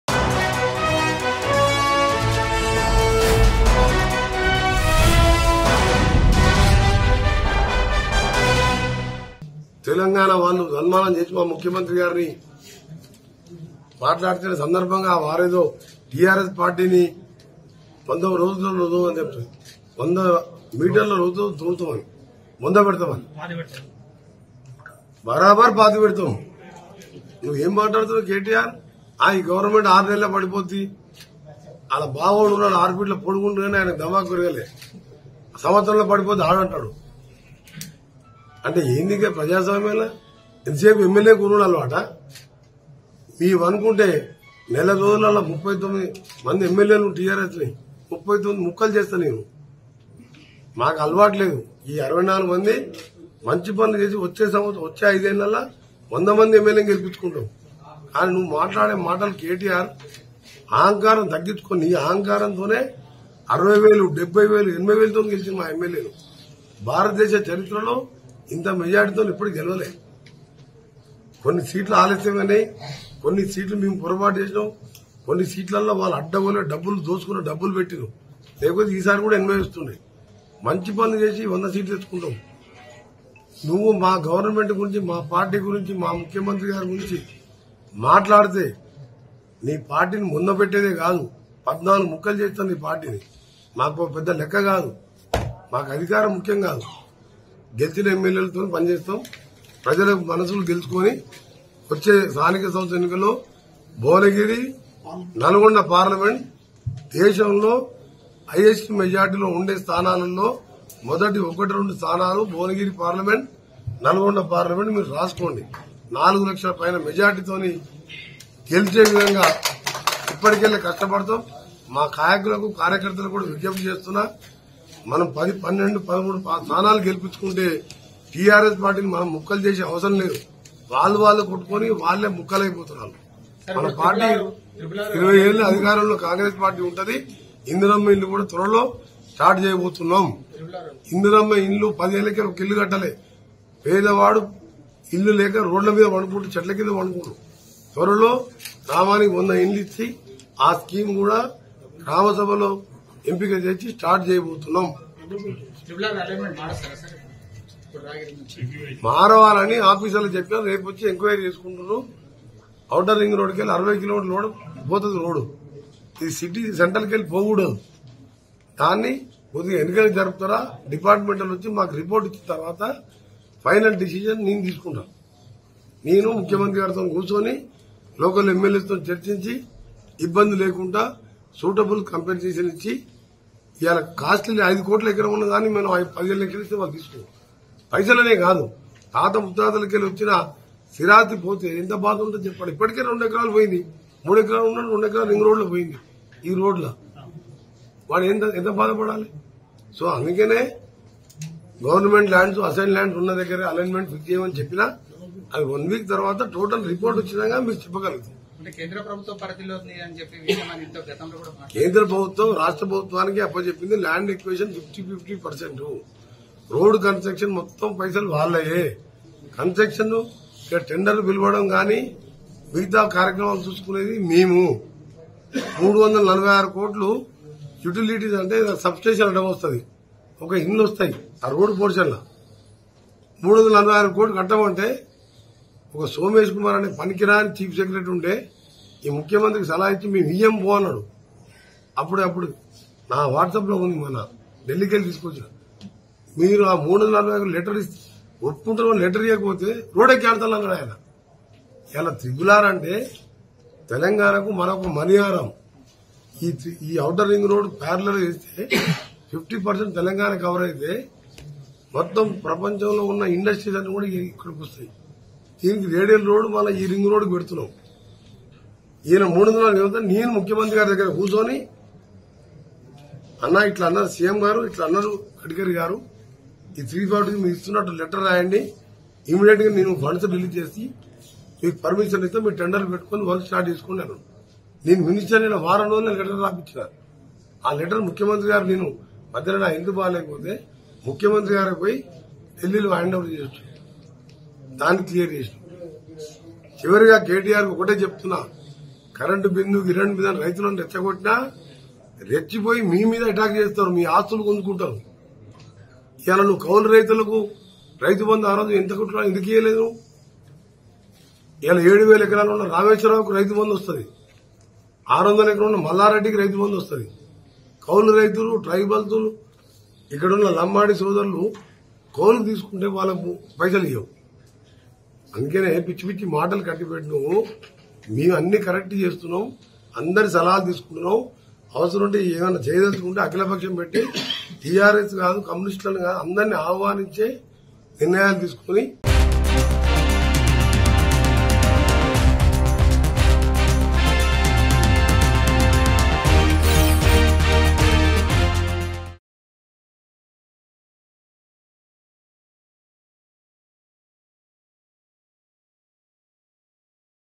Minister Komatireddy Venkat Reddy Says About Regional Ring Road Alignment | Telangana | Mango News
Minister Komatireddy Venkat Reddy Says About Regional Ring Road Alignment.